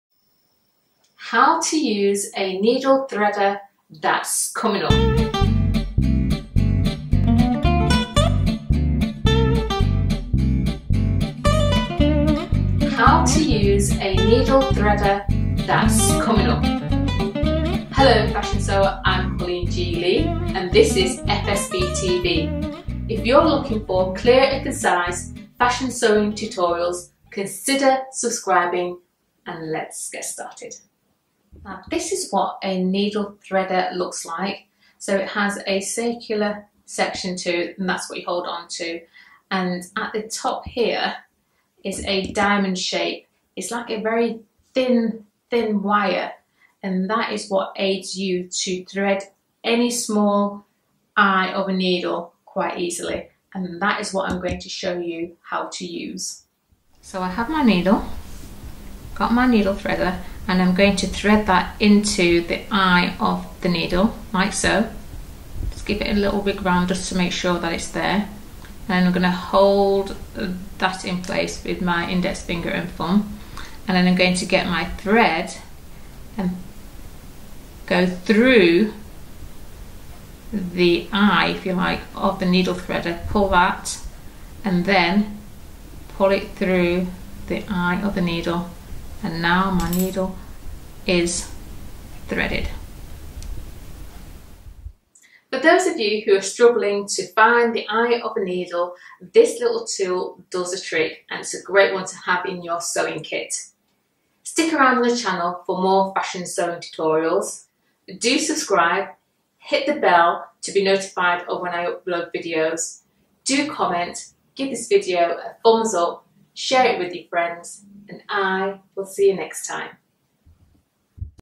How to Use a Needler Threader
How to Use a Needler Threader. Threading the eye of a needle can be frustrating, there is an amazing little tool to make the process a lot simpler, I demonstrate the tips and give advice about needle threaders and how to use them correctly.

Supplies Used In The Video

Needle Threader